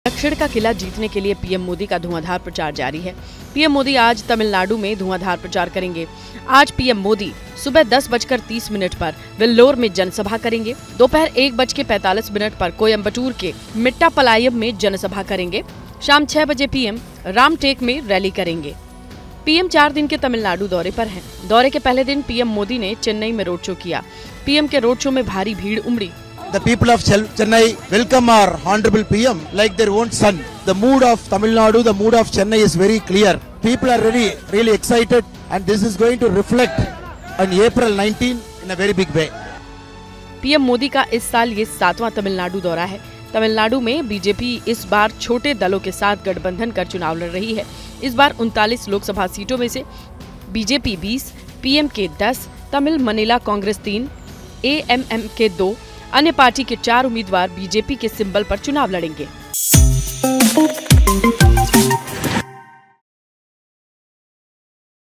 दक्षिण के किले पर PM Modi फूकेंगे चुनावी बिगुल, ‘400 पार’ को लेकर भरेंगे हुंकार | ABP LIVE
News: मिशन 400 पार मे जुटे पीएम मोदी धुआंधार चुनाव प्रचार कर रहे हैं. आज पीएम मोदी तमिलनाडु और महाराष्ट्र में चुनाव प्रचार करेंगे...
पीएम आज तमिलनाडु के बेल्लोर और कोयंबटूर में जनसभा करेंगे...शाम को पीएम महाराष्ट्र  के रामटेक में रैली करेंगी...मंगलवार को चेन्नई पहुंचने के बाद पीएम मोदी ने रोड शो किया...पीएम के रोड शो में भारी भीड़ उमड़ी  |